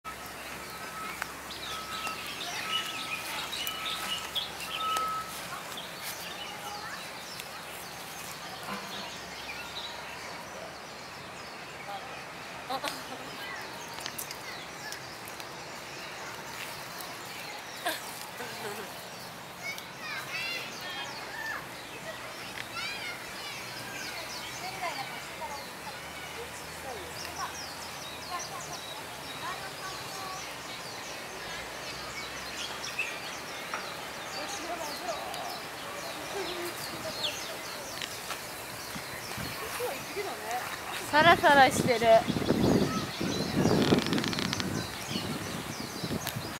Hawaii 04/04/2011 Honolulu Zoo Part 5 by SONY DSC-HX9V
Hawaii 04/04/2011 Honolulu Zoo Part 5

by SONY DSC-HX9V
ホノルルにある動物園です。日本では見ることの出来ない珍しい動物も多々。

SONY DSC-HX9Vで撮影しました。